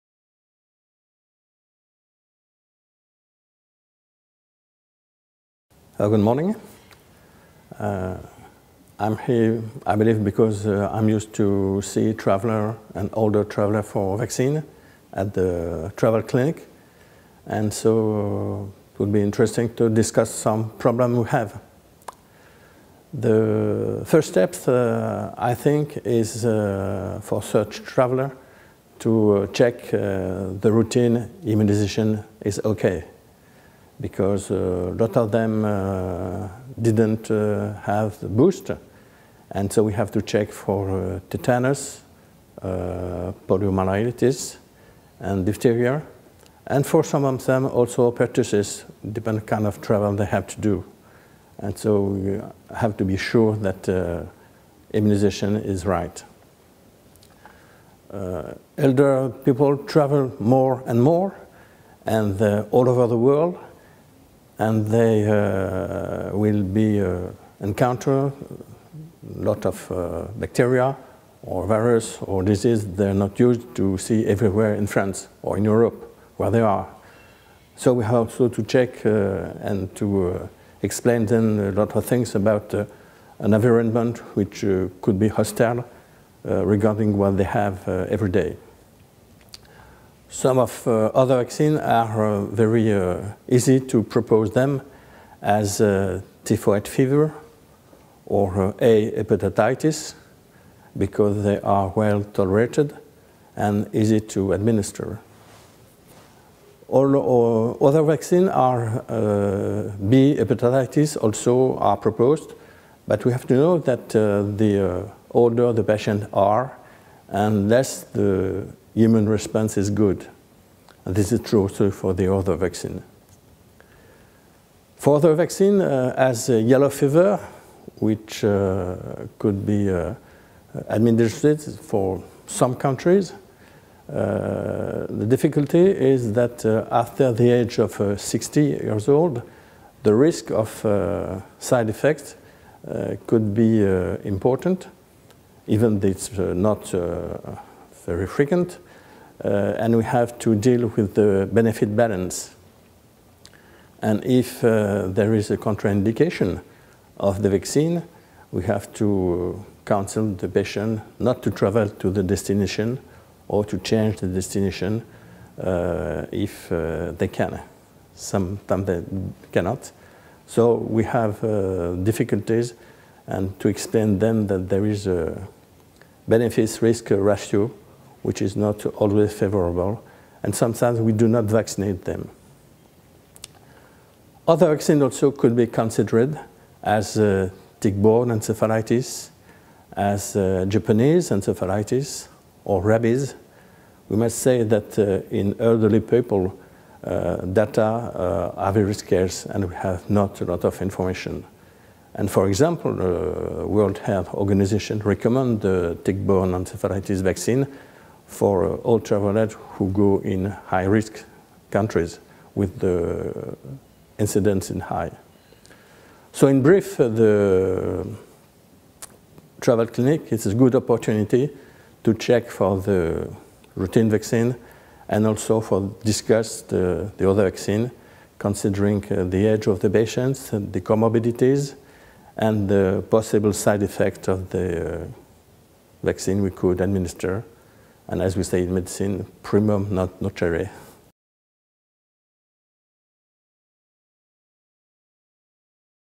Vaccines for the traveller elderly - Christian Chidiac
Nowadays, elderly travel more frequently and longer distance than in the past and in these cases they are often exposed to lot of bacteria they are not used to in their home countries.
Before leaving on a trip,  prof. Chidiac (Lyon, France) suggests, they should check that the routine immunization is ok (tetanus, poliomyelitis, difteria, and sometimes pertussis) and then discuss with the physician the best immunization strategy: some vaccines are well tolerated and easy to administered (typhoid fever, A hepatitis) but in some other cases the older the patient, the less the immune response or the riskier the side effects and therefore it is necessary to evaluate the benefit balance.